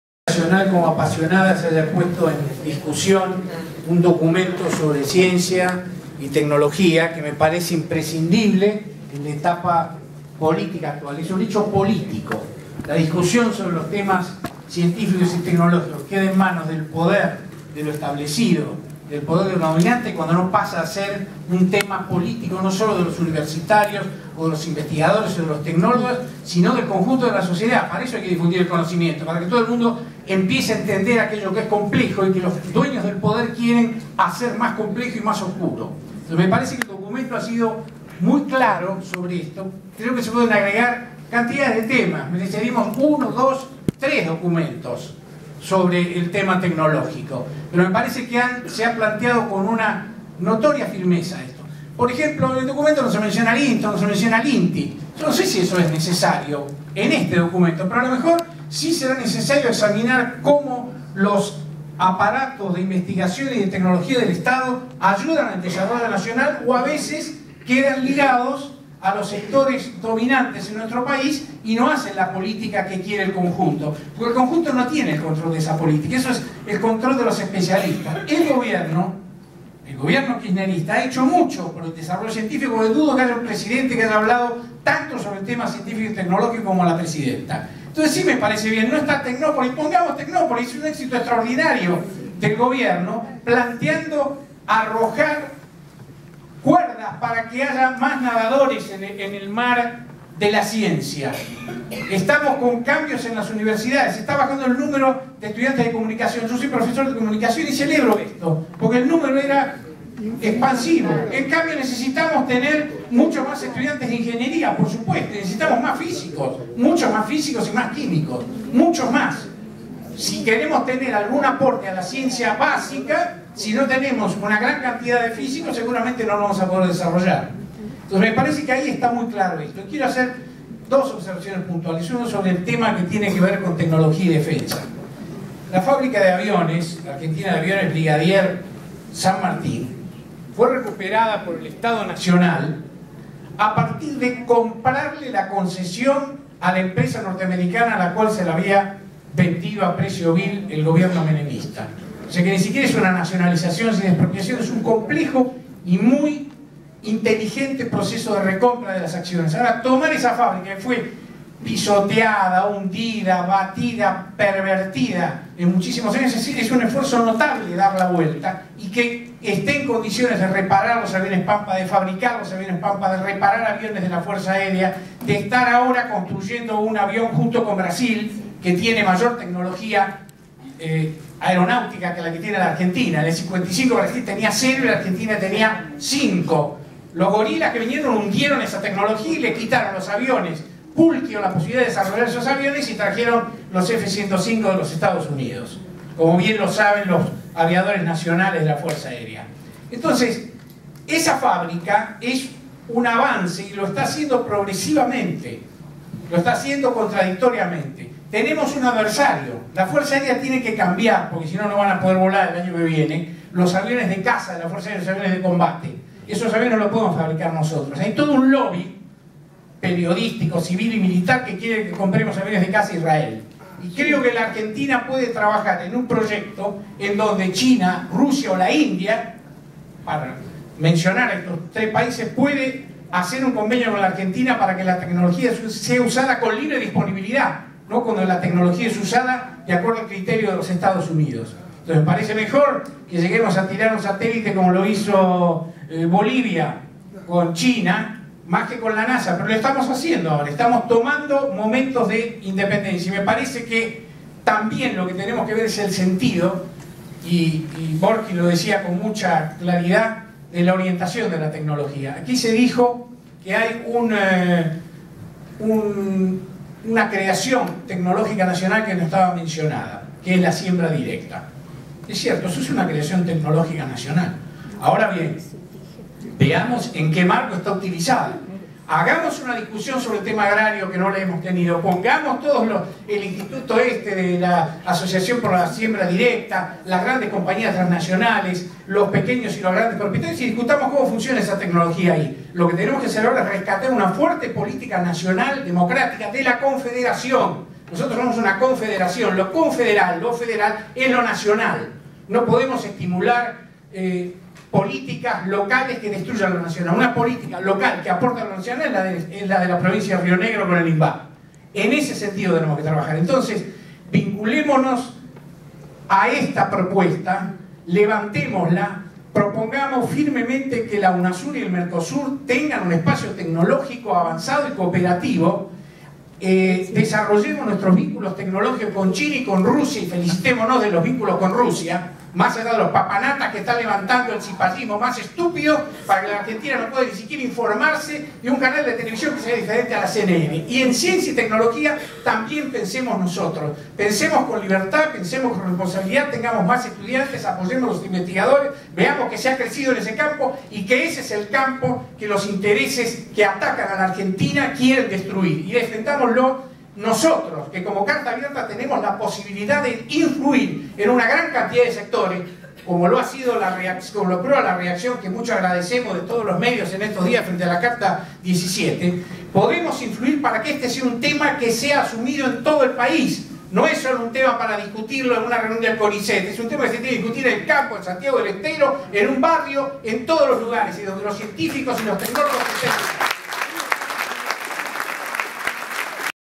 09CA Jorge Bernetti / Tecnología
Discusión del documento "Proyecto Nacional de Desarrollo Inclusivo. Su Estilo Tecnológico", elaborado por la Comisión de Desarrollo Tecnológico de Carta Abierta. Asamblea del 11 de octubre de 2014. Biblioteca Nacional. Buenos Aires.